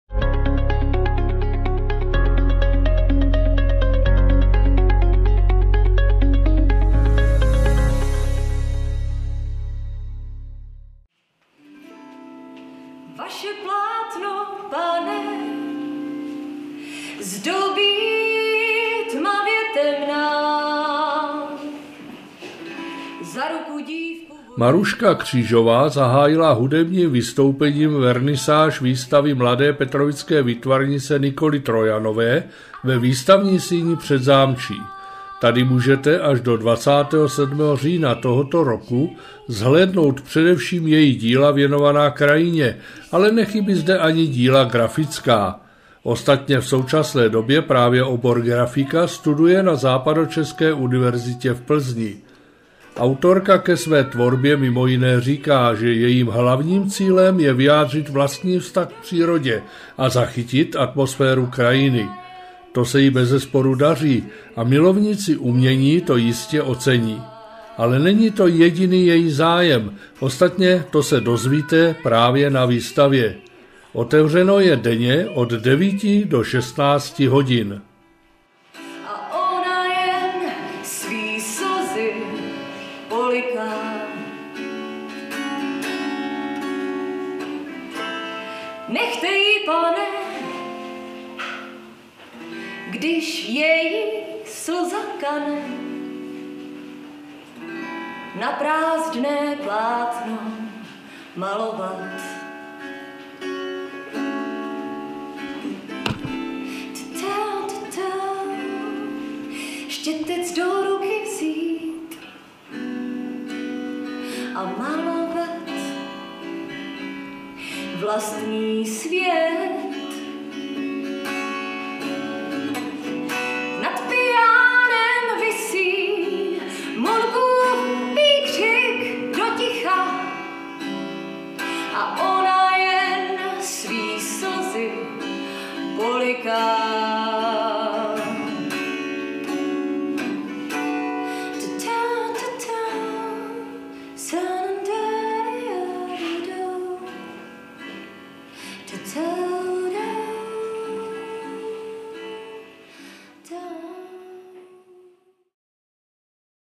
Umění v krajině Nikoly Trojanové
Ve Výstavní síni Předzámčí probíhá výstava obrazů mladé talentované výtvarnice Nikoly Trojanové...